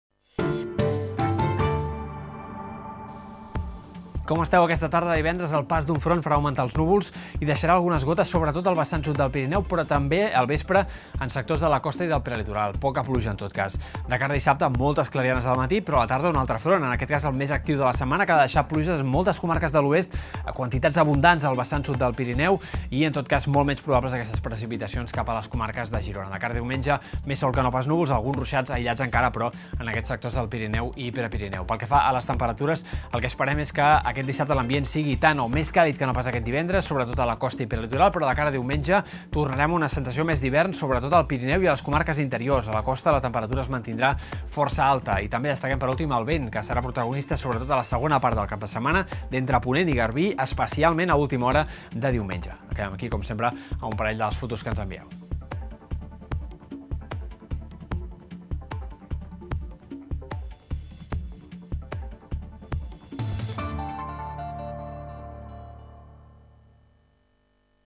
La méteo en 1 minut: sol, estones de pluja i canvis de temperatura
El cap de setmana començarà amb regust de primavera però acabarà amb sensació d'hivern.